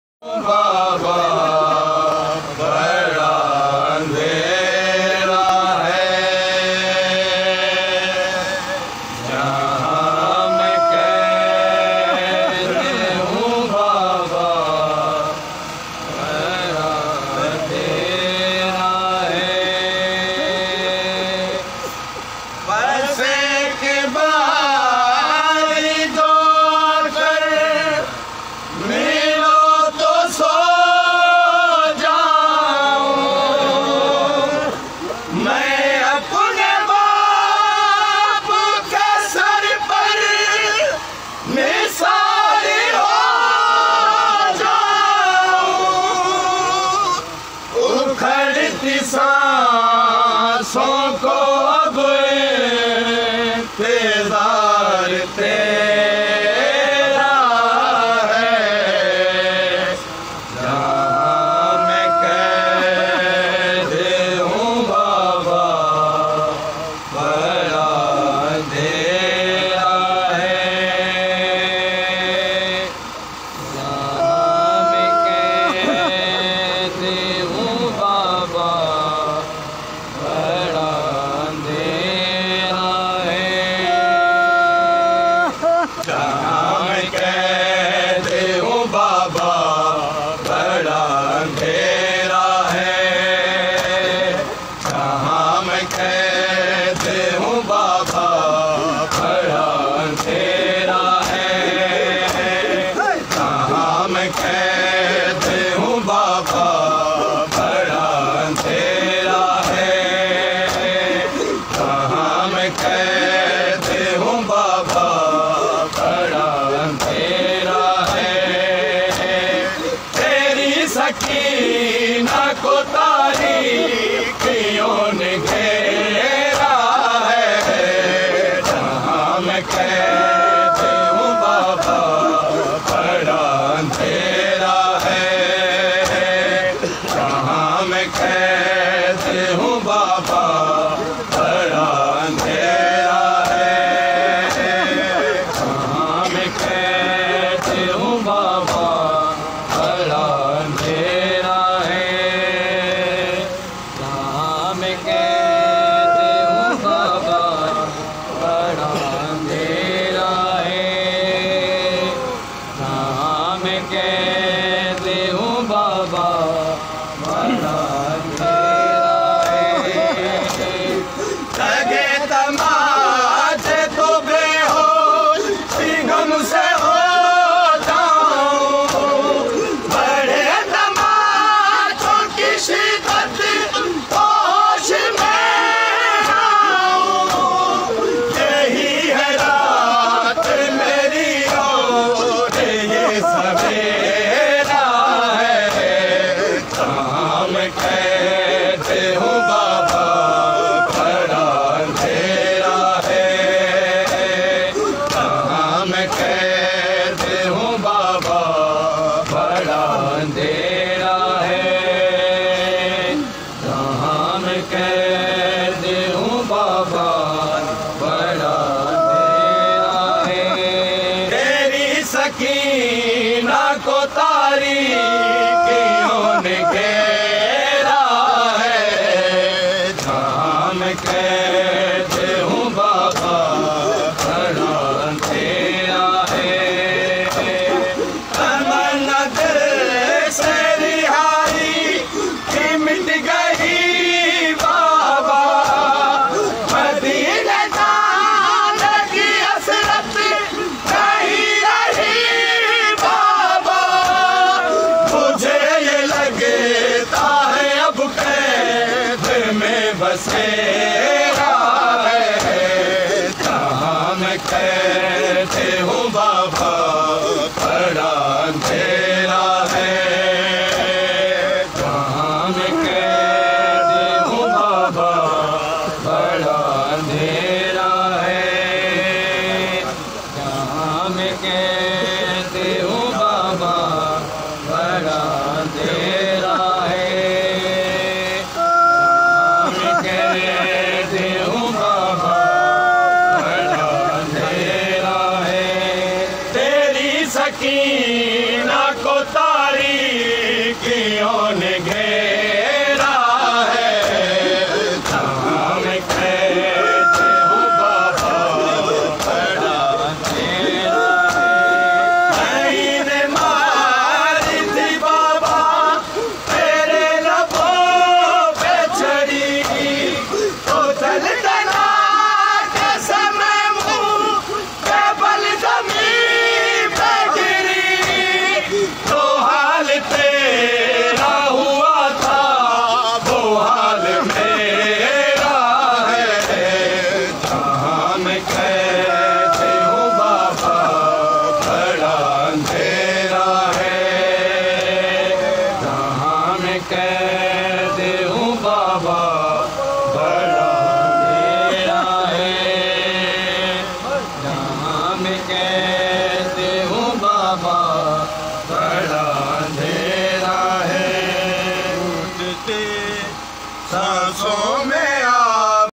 نوحا خوان: استاد اطهر علي ميراڻي - سوڀوديرو
محرابپور: ماتمي جلوس بسلسله شهادت بي بي سڪينه سلام الله عليها 10 صفر المظفر 1444ھ - 7 سيپٽمبر 2022ع